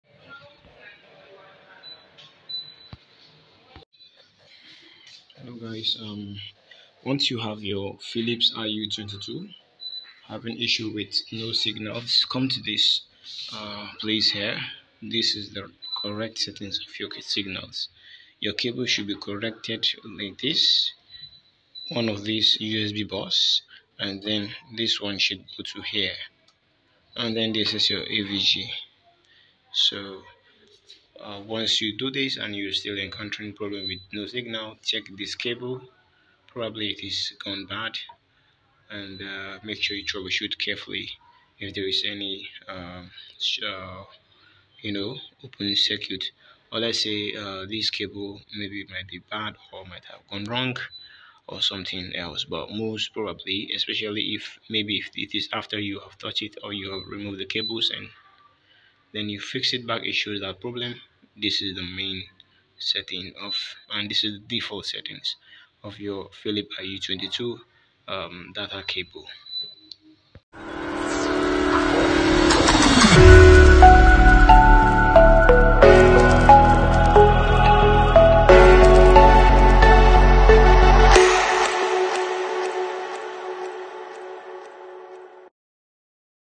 Philip IU-22 Ultrasound showing no signal Solution
If your Philip IU-22 Ultrasound is showing no signal, don't worry! Watch this video for a simple solution to fix the issue. Get your ultrasound back up and running in no time!